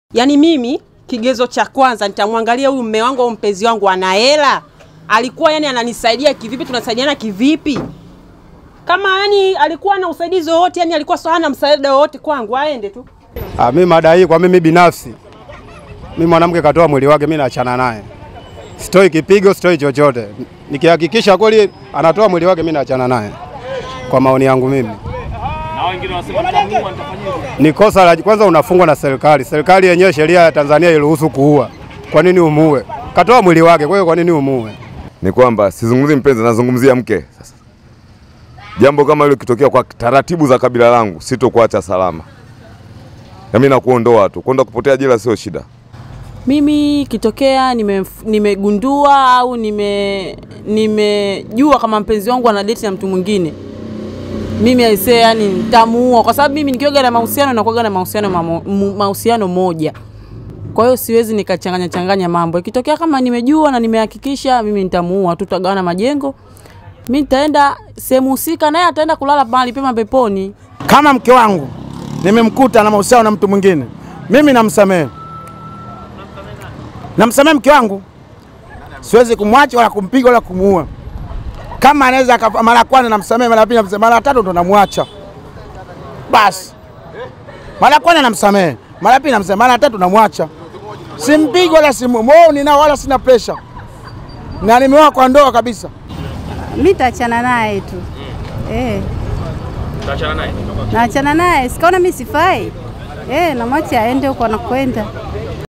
NIKIMFUMANIA MPENZI WANGU NAMUUA
WAKAZI WA JIJI LA DARES SALAAM WAKITOA MAONI YAO KUHUSU SUALA ZIMA LA WIVU WA MAPENZI NA USALITI ,MARA BAADAYA KUTOKEA MATUKIO YA MARA KWA MARA YA MAUAJI YANAYOELEZWA YANASABABISHWA NA WIVU WA MAPENZI NA USALITI